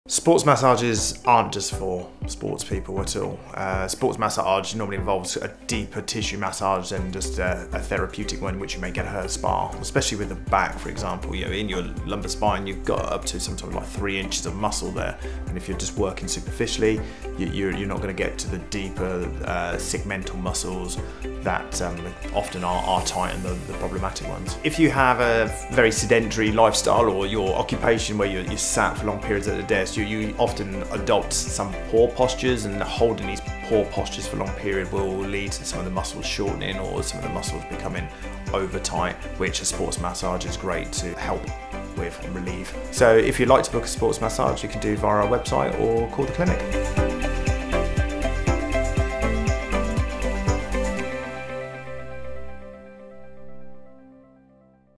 Why bother with a Sports Massage?
If you assume Sports Massage is just for Sports people then think again.

Sports Massage is for anyone - any age - any ability.

Watch my short video to find out more about what it is and how it can help you.